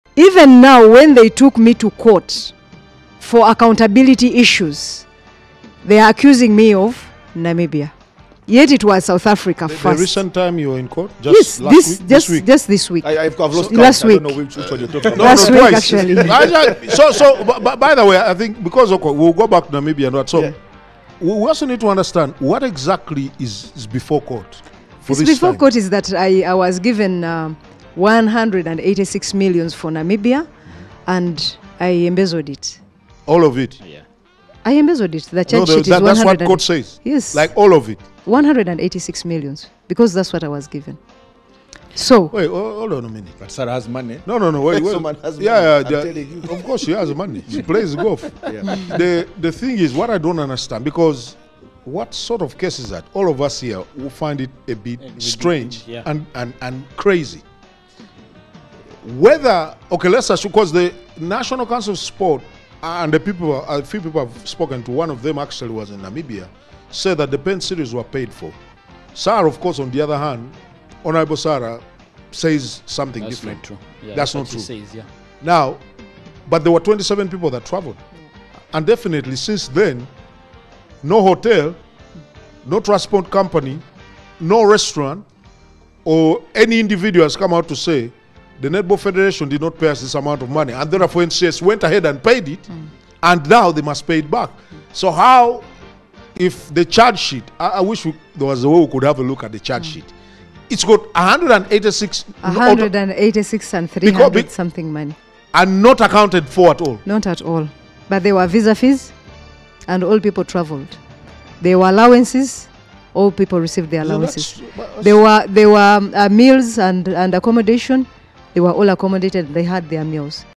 President of Uganda Netball Federation Sarah Kityo on the allegations of corruption
MBL production